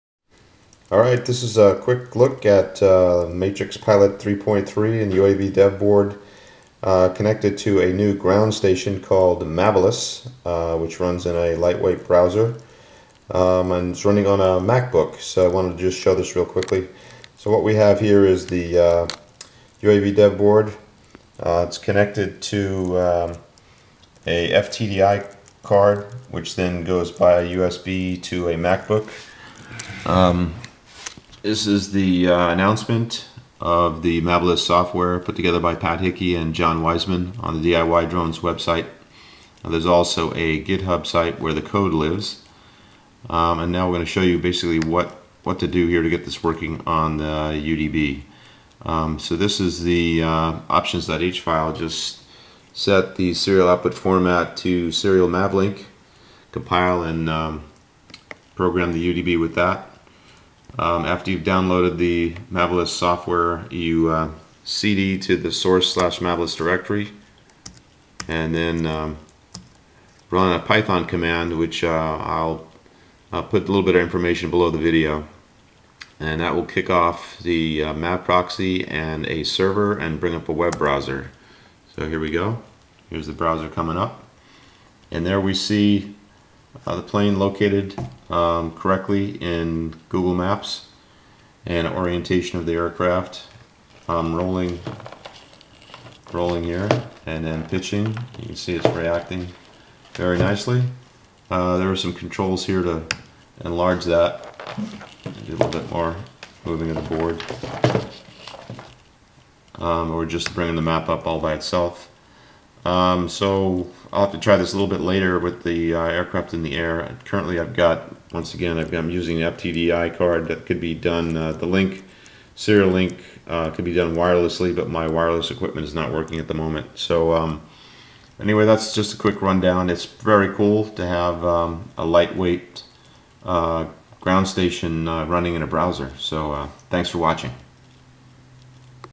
UDB4, MatrixPilot 3.3 and Mavelous Ground Station w/MavProxy running on a MacBook
A short demo of the Mavelous Ground Station running on a MacBook. The IMU board is the UAV Dev Board (UDB4) running MatrixPilot 3.3 autopilot software.

Here are 2 terminal window commands used in the video to launch Mavelous:

cd ~/dev/UAV/mavelous/src/mavelous

python mavproxy.py --master=/dev/tty.usbserial-A600exCq --baud=57600 --module mmap

See the following links for more information about the UDB, MatrixPilot and Mavelous.